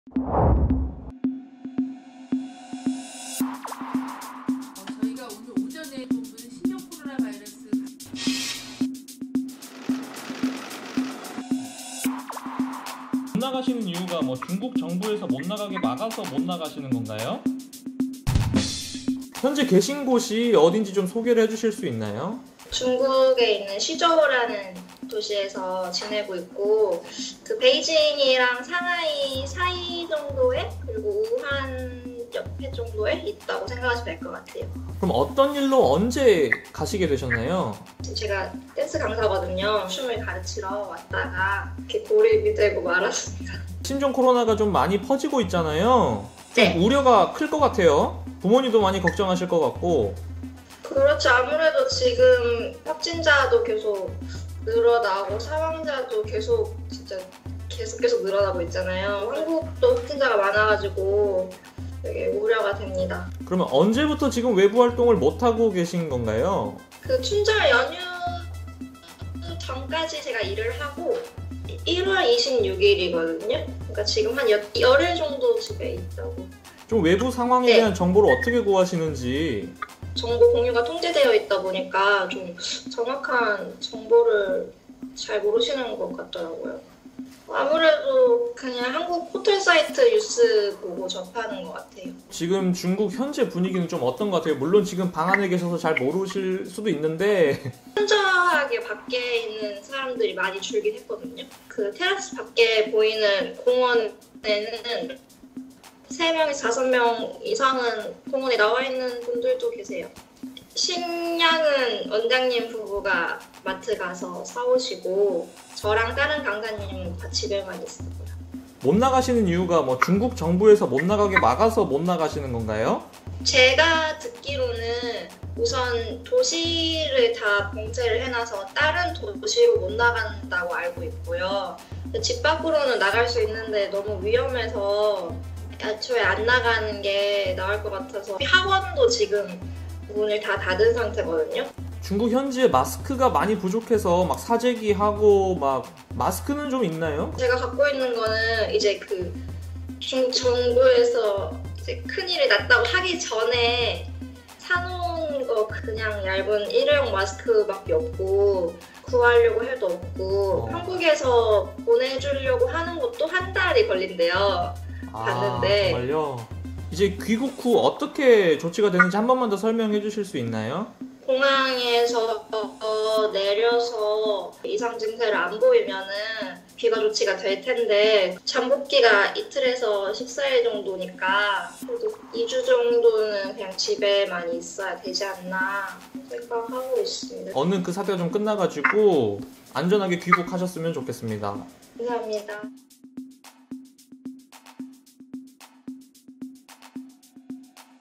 신종 코로나로 중국에 고립된 한국인과의 영상통화.. 현지 상황 물어봤습니다. | 두둥탁 ep.15
신종 코로나바이러스가 확산세입니다. 한국에서만 확진자가 23명이나 발생했죠. 이런 와중에 중국 현지에서 고립된 한국인과 연락이 닿아 이야기를 나눠볼 수 있었습니다.